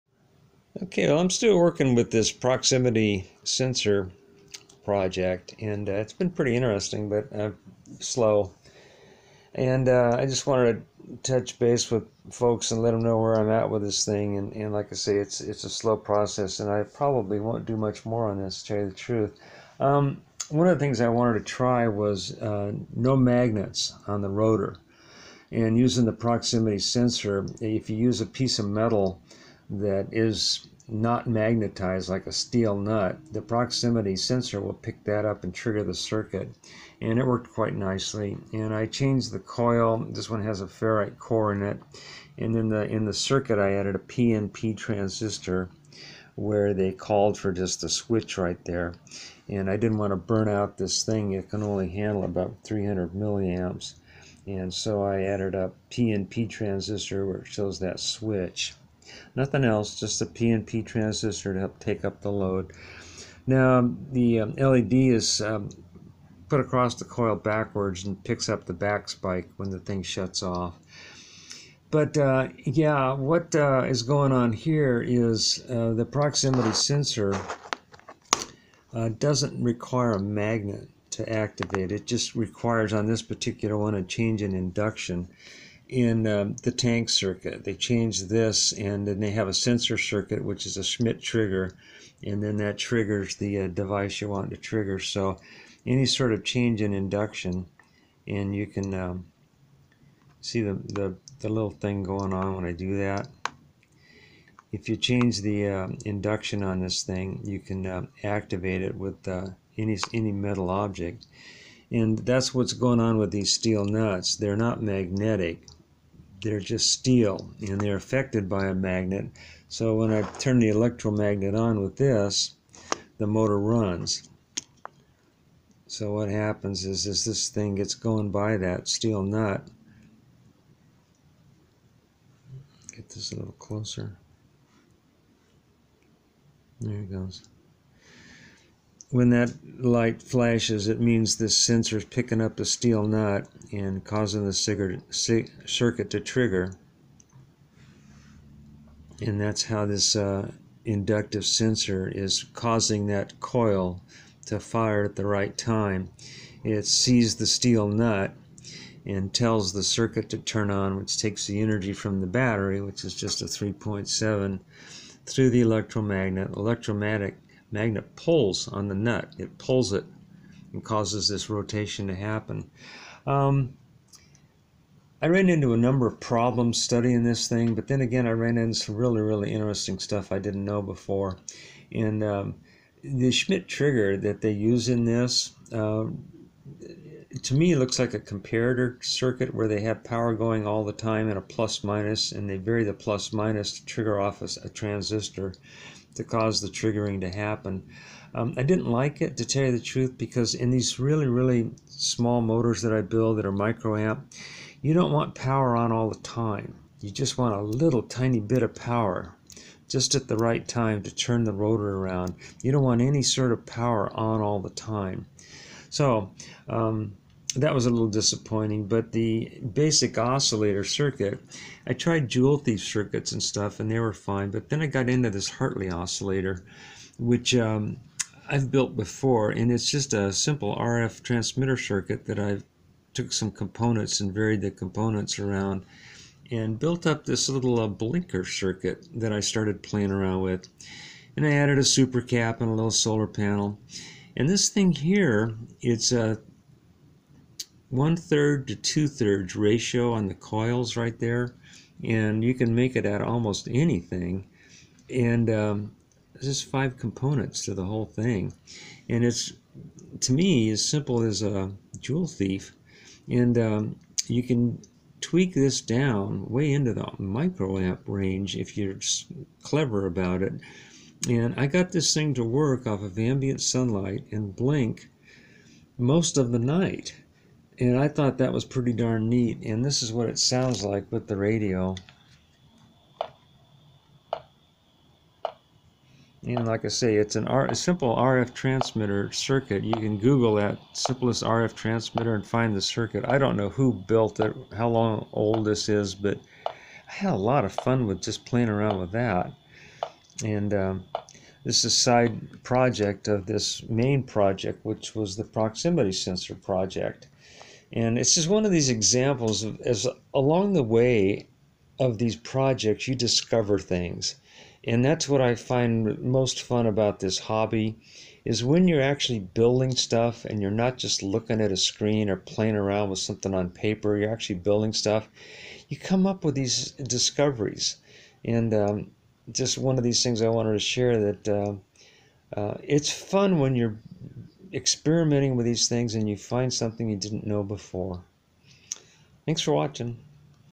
Pure Attraction Motor --- using inductive proximity sensor
This is a simple homemade pulse motor that uses steel nuts on the rotor instead of magnets.  An electromagnet pulls on the rotor steel nut as they go by and causes rotation.  The triggering is done by an inductive type of proximity sensor.  I also show a very simple oscillator circuit that I am studying in an effort to build my own proximity sensor.  My homemade proximity sensor quest has not been successful but I have learned a few new things along the way.